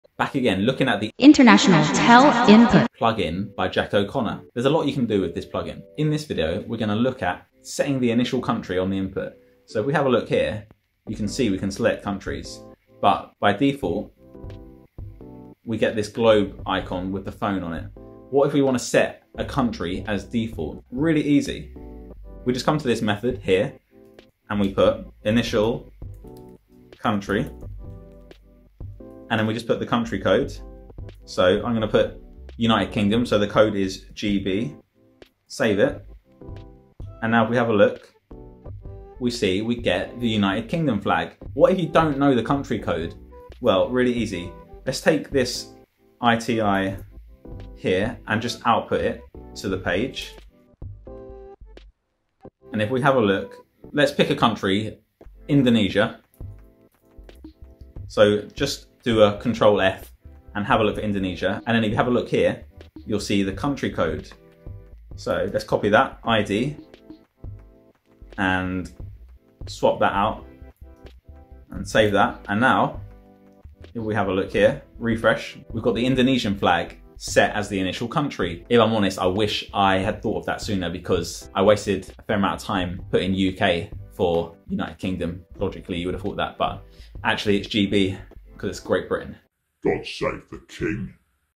intl-tel-input Set Initial Country Flag [VUE.JS]
How to set the initial country flag when using the intl-tel-input plugin in Vue.js. We do this by setting "initialCountry" to the country code for the country we wish to show when the page loads. In this example I show you how to do this in Vue.js.